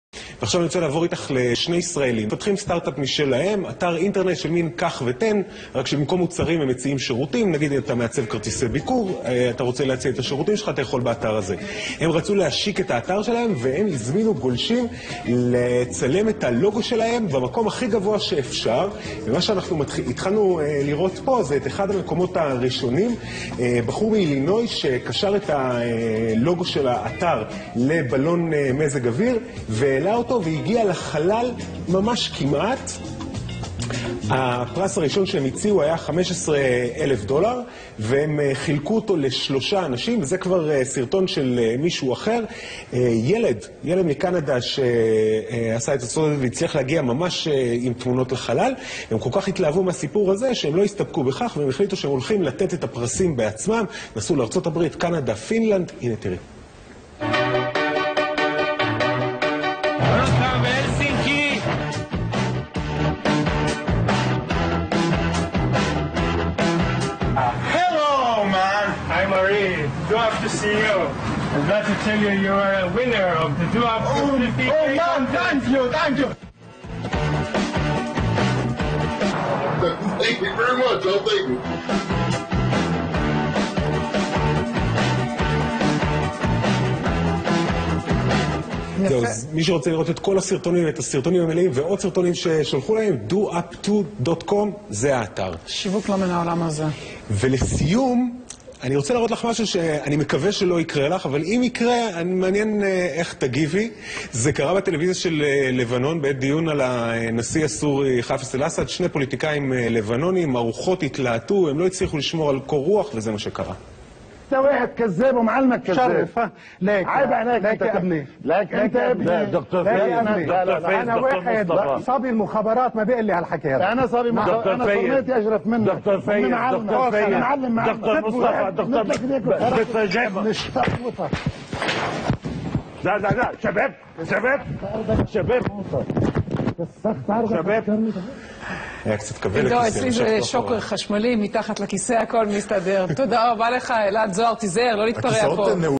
DoUpTo Campaign on Channel 2 קמפיין דואפטו בערוץ
קמפיין דואפטו שבמסגרתו לוגו החברה הוטס לחלל, בערוץ 2